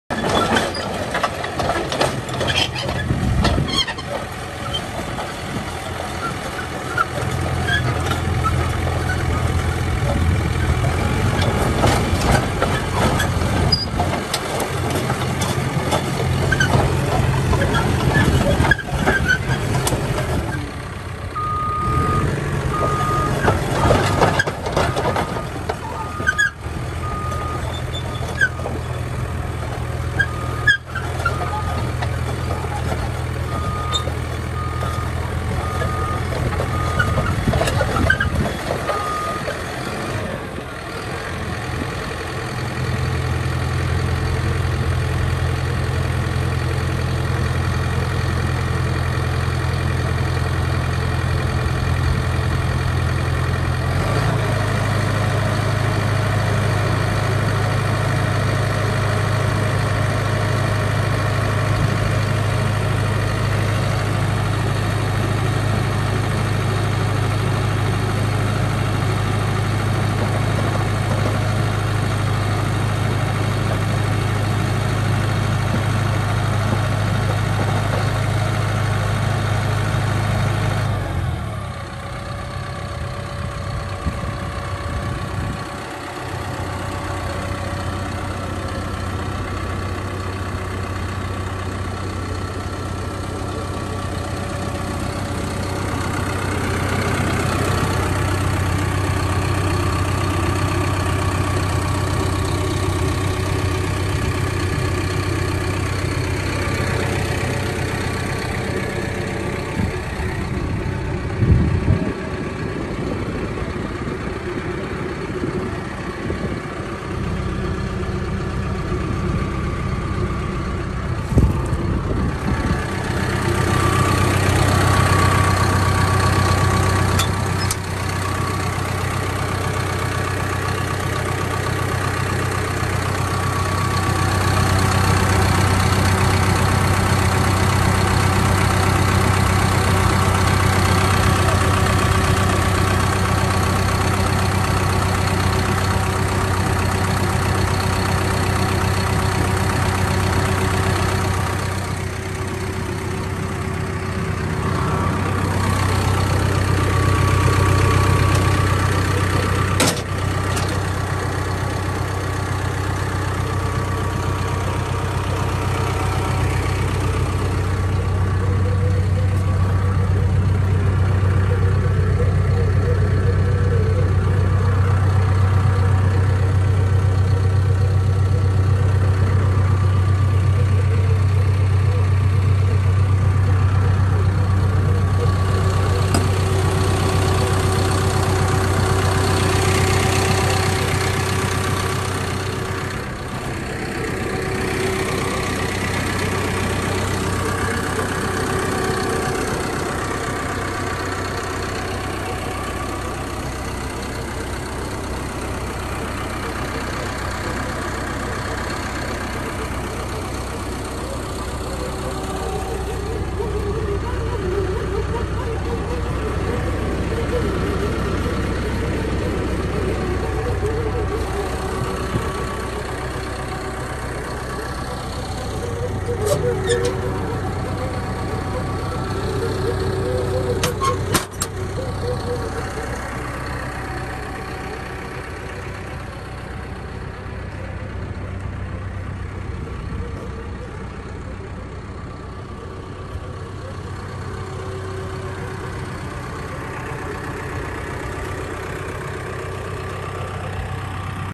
2015 CATERPILLAR D3K2 XL For Sale
4 CYL CAT ENGINE, HYDRO-STATIC TRANS, 16 IN PADS, 8 FT 6 IN 6-WAY BLADE, JOYSTICK STEERING, OROPS